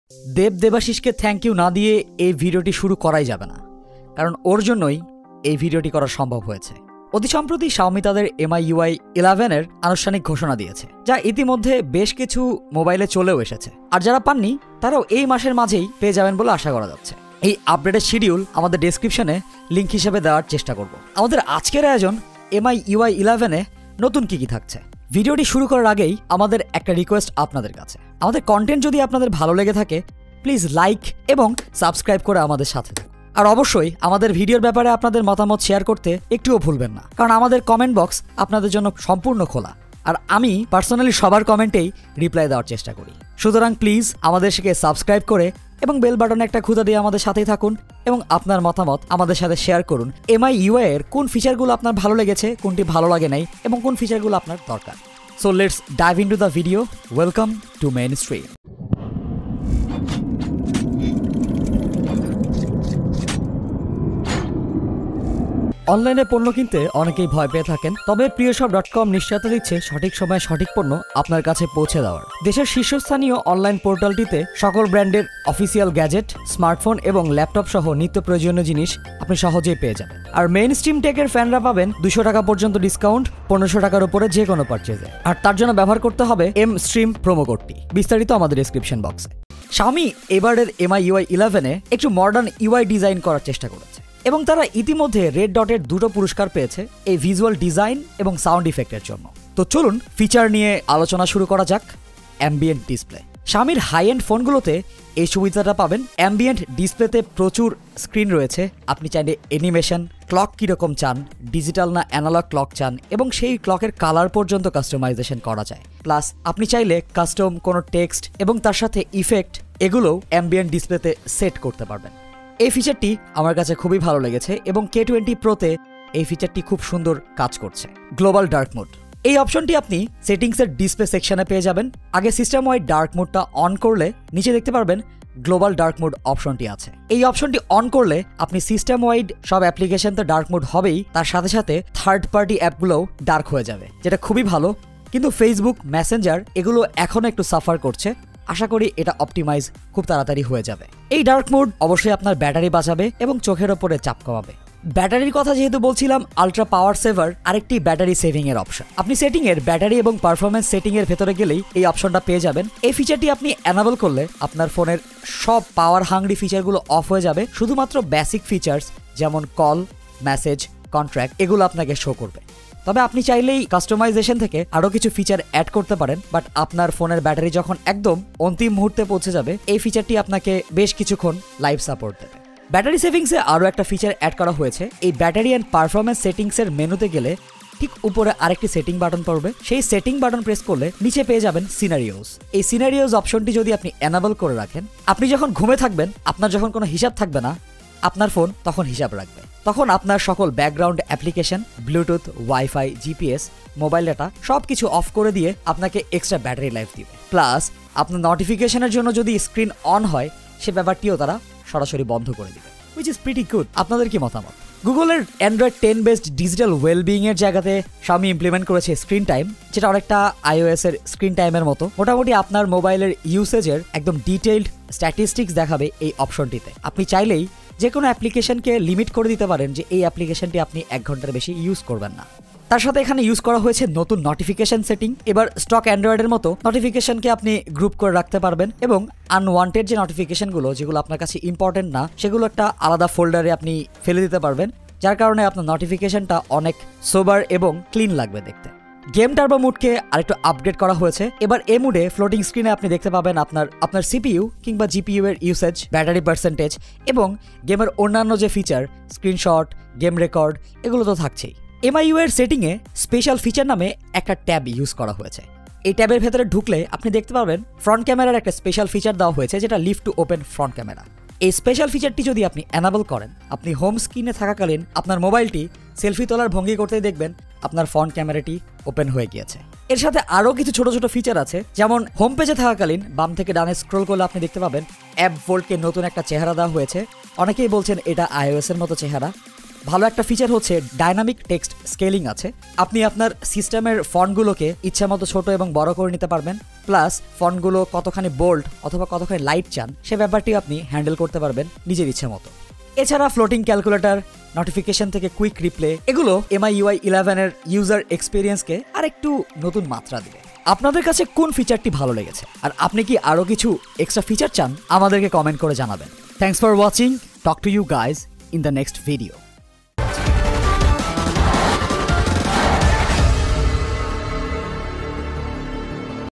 Xiaomi MIUI 11 - First Look & New Features (Bangla)
Xiaomi MIUI 11 - First Look & New Features is live on our channel now.  Xiaomi MIUI 11 based on Android 10 Q is now OFFICIAL. Here's our Full Review of MIUI 11 from Xiaomi running on Redmi K20 / Redmi K20 Pro.

MIUI 11 Features, Design, Interface, UI, Settings and Everything you need to know.

PriyoShop.com is an online shopping destination for Bangladesh offering a completely hassle-free shopping experience through secure and trusted gateways.